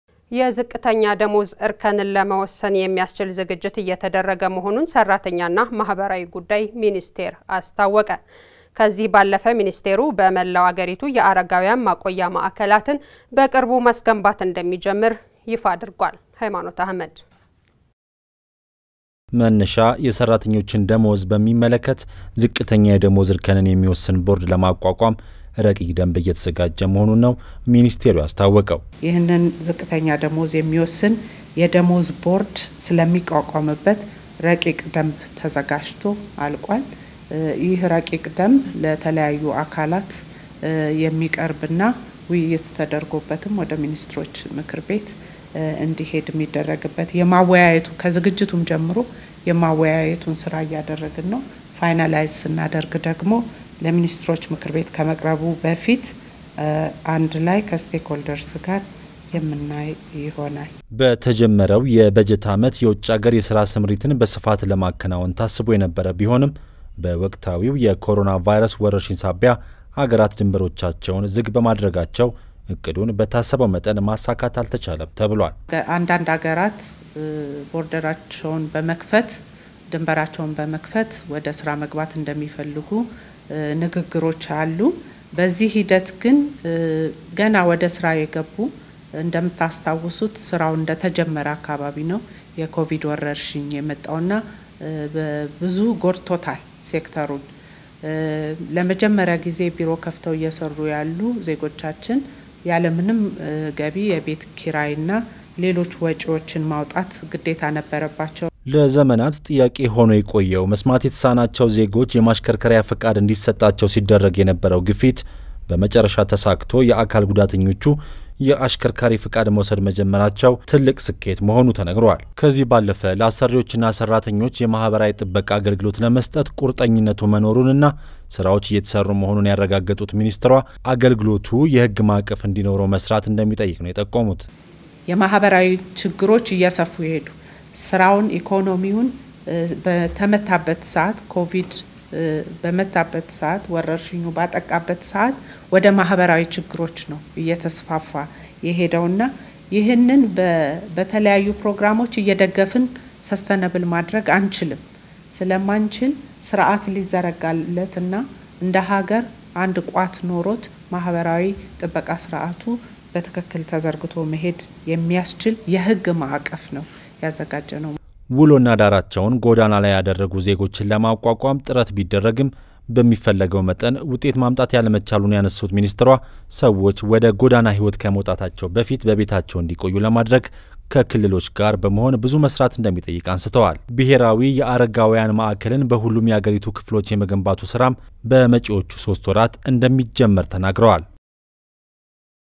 ዝቅተኛ የደመወዝ እርከን
የዝቅተኛ ደመወዝ እርከንን ለመወሰን የሚያስችል ዝግጅት እየተደረገ መሆኑ ተነገረ፡፡